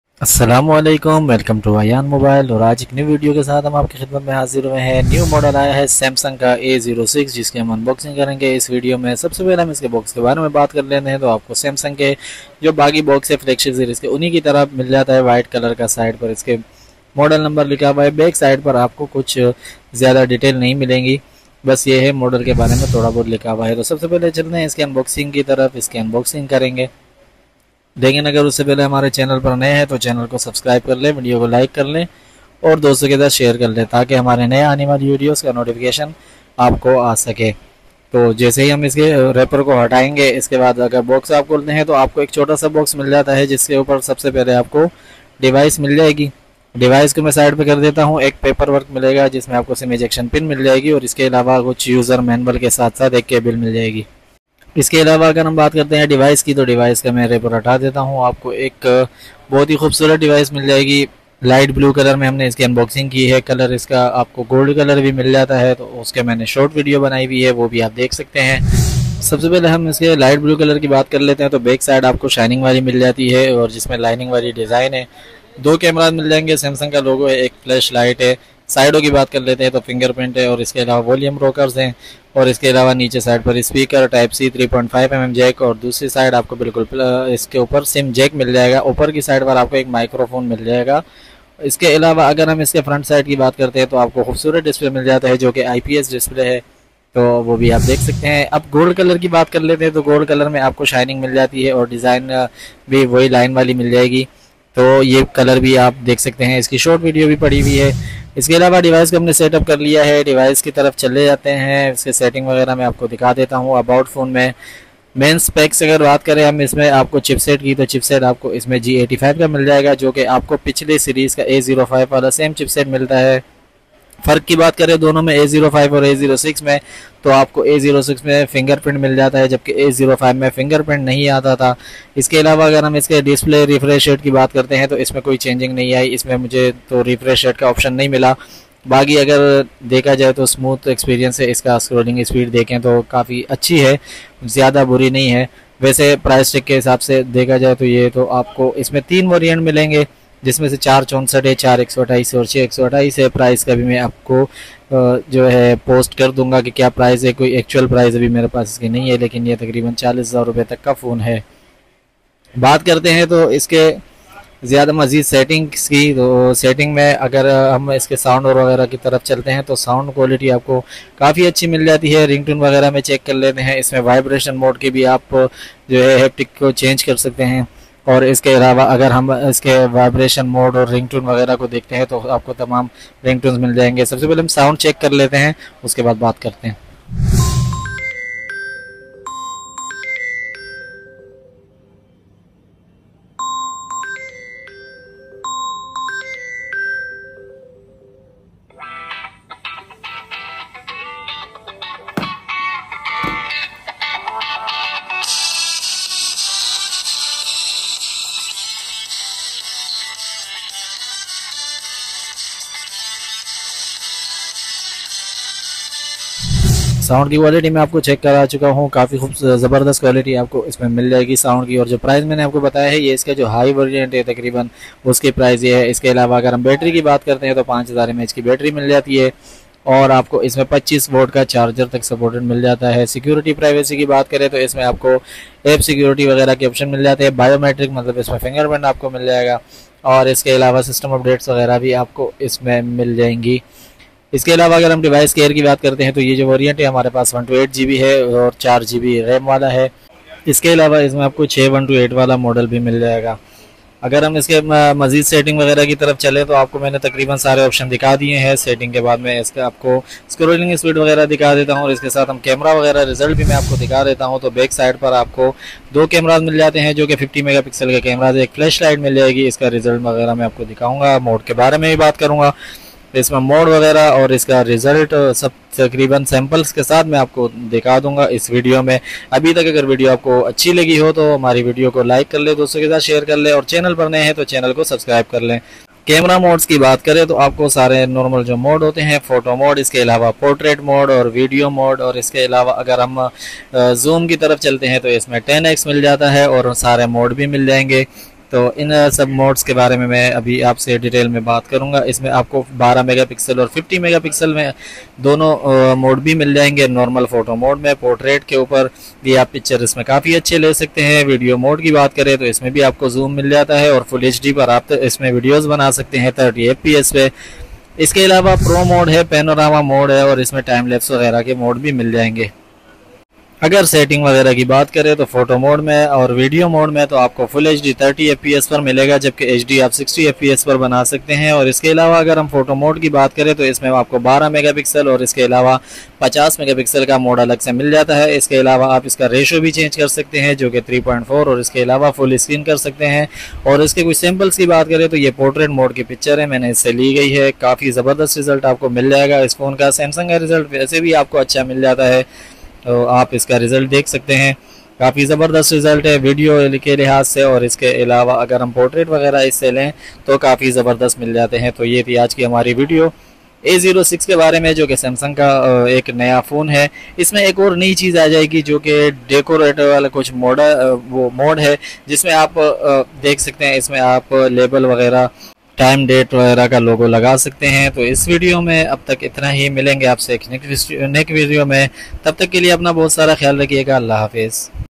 “Samsung Galaxy A06 Unboxing & Review – Affordable Performance in Pakistan!”
In this video, we unbox and review the Samsung Galaxy A06, a budget-friendly smartphone packed with impressive features. From its sleek design to its solid performance, we take an in-depth look at its camera quality, display, and battery life, providing an honest review to help you decide if the Galaxy A06 is the right choice for you.

Be sure to check out my TikTok for more unboxings and reviews: Ayan Mobile TikTok.